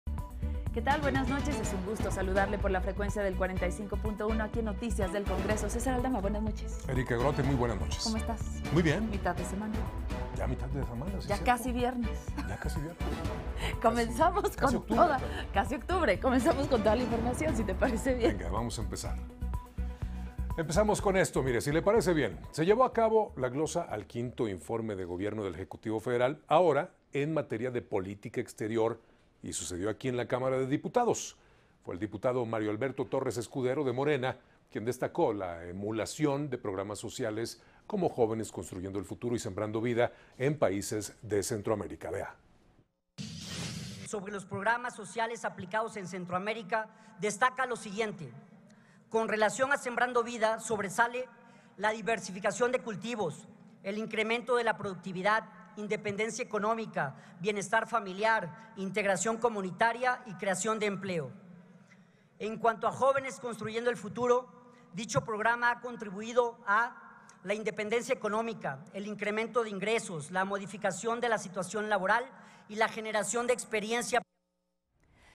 Analizan política exterior en San Lázaro
Se llevó a cabo la glosa al Quinto Informe de Gobierno del Ejecutivo Federal en materia de política exterior en la Cámara de Diputados. Fue el diputado Mario Alberto Torres Escudero, de Morena, quien destacó la emulación de programas sociales como "Jóvenes construyendo el futuro" y "Sembrando vida", en los países de Centroamérica.